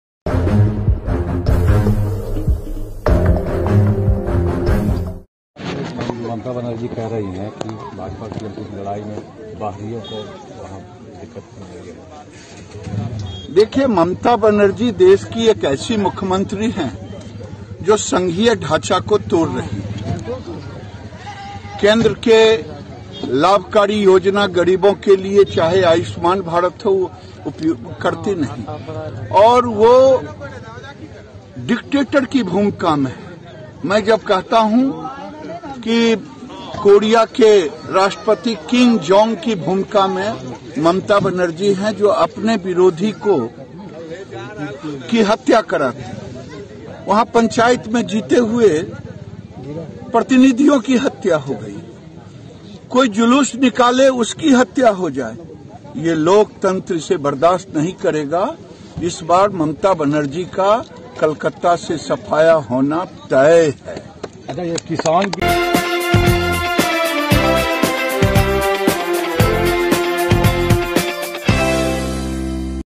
Mamta Banerjee as Dictator likeKing Jong : Giriraj Singh Central Minister | All Rights
अपने बेबाक बयानों के लिए मशहूर भाजपा के कद्दावर नेता और केन्द्रीय मंत्री गिरिराज सिंह (Giriraj Singh) एक बार सुर्खियों में हैं. इस बार उन्होंने पश्चिम बंगाल की मुख्यमंत्री ममता बनर्जी (Mamata Banerjee ) पर बड़ा बयान दिया है. गिरिराज सिंह ने पश्चिम बंगाल की मुख्यमंत्री की तुलना उत्तर कोरिया के तानाशाह किम जोंग उन से कर दी है.

बता दें कि केंद्रीय मंत्री गिरिराज सिंह शुक्रवार को लखिसराय के बड़हिया स्थित अपने पैतृक गांव पहुंचे. इस दौरान केंद्रीय मंत्री ने पत्रकारों के सवालों का जवाब दिया. केंद्रीय मंत्री गिरिराज सिंह ने पश्चिम बंगाल पर जोरदार हमला बोला. गिरिराज सिंह ने ममता बनर्जी की तुलना उत्तर कोरिया के तानाशाह किम जोंग से की. केंद्रीय मंत्री ने कहा कि ममता बनर्जी अपने विरोधियों की हत्या की राजनीति करती हैं. कोई जुलूस निकाले तो उसकी भी हत्या हो जाती है. इस बार बंगाल के लोग इसे बर्दाश्त नहीं करेंगे और ममता बनर्जी का बंगाल से जाना तय है. वे पूरी तरह से डिक्टेटर की भूमिका में हैं.